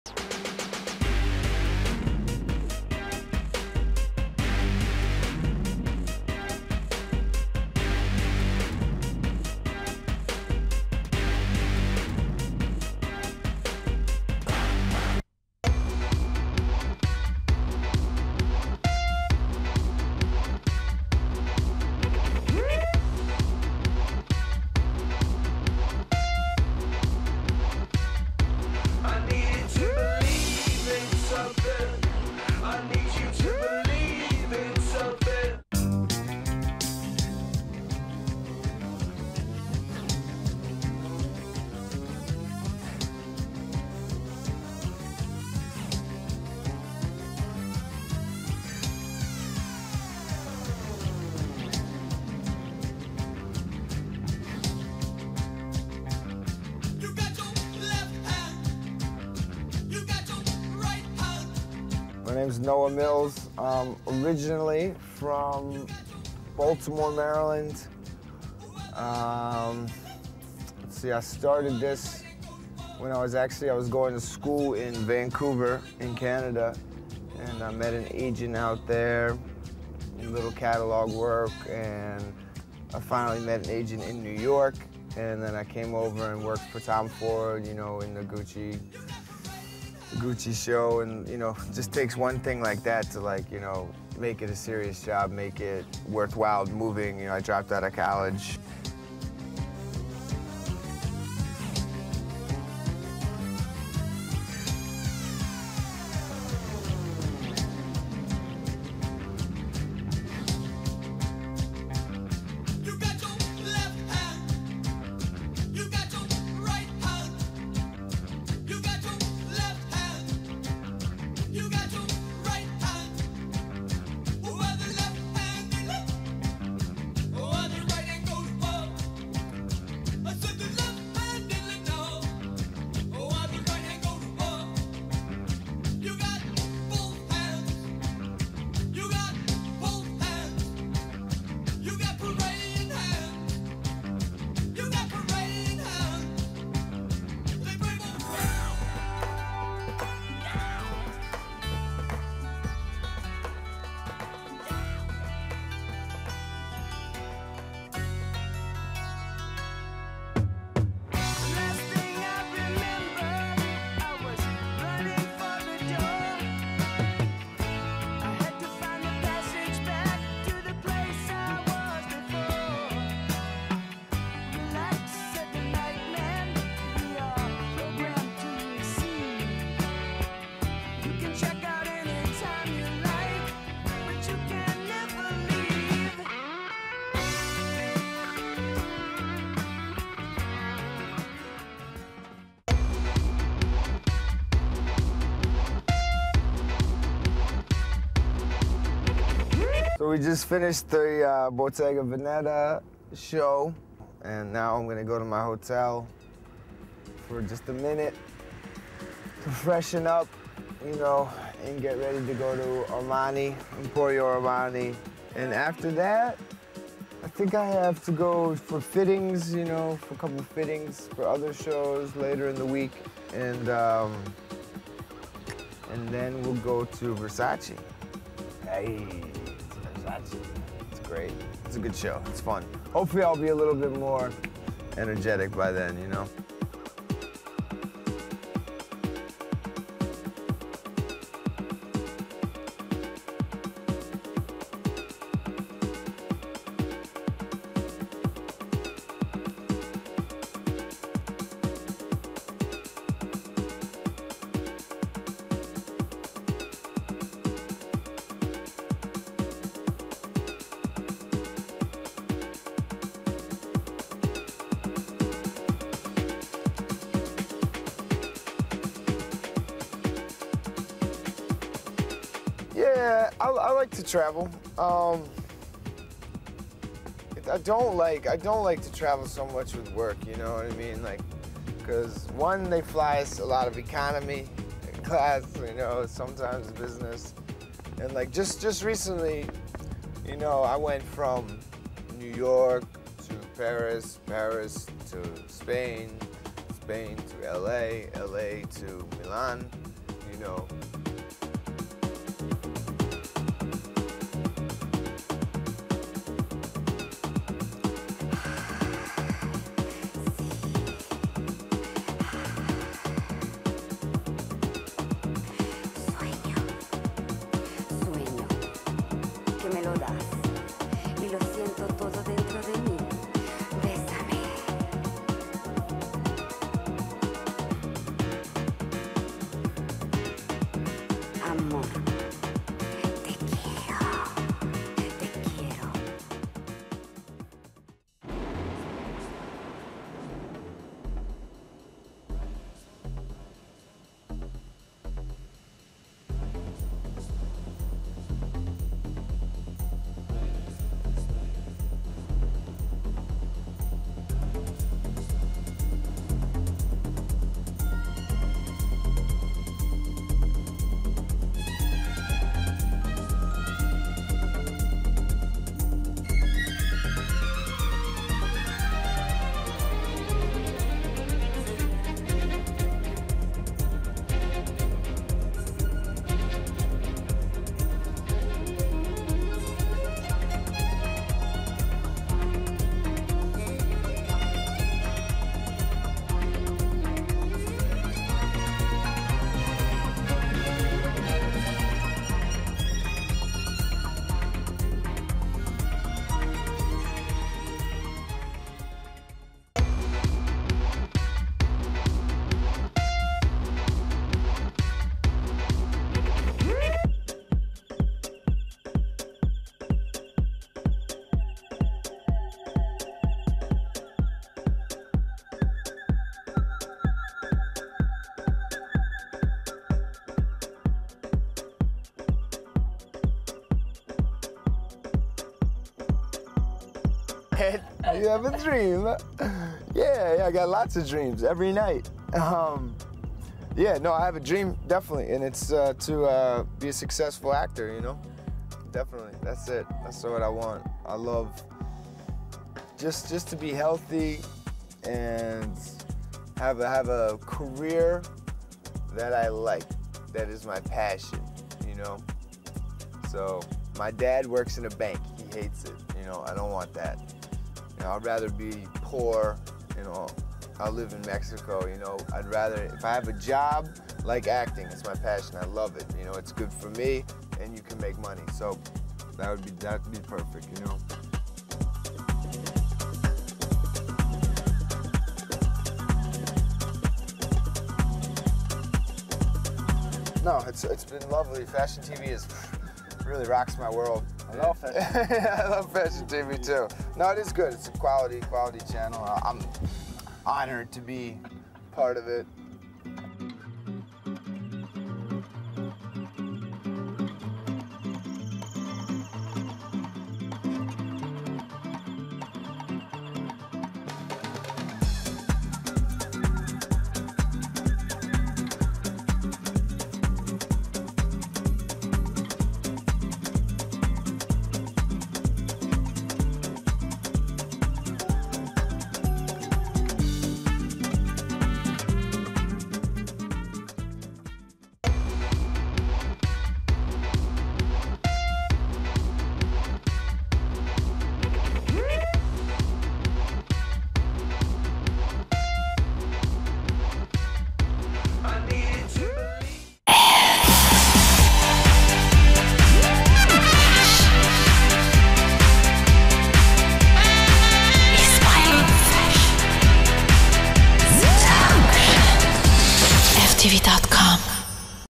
fashiontv | FTV.com - NOAH MILLS MODEL 24/7 P/E 2007
Download the FashionTV+ app, and stay tuned:
Take a tour backstage with Noah to view Milans Fashion Week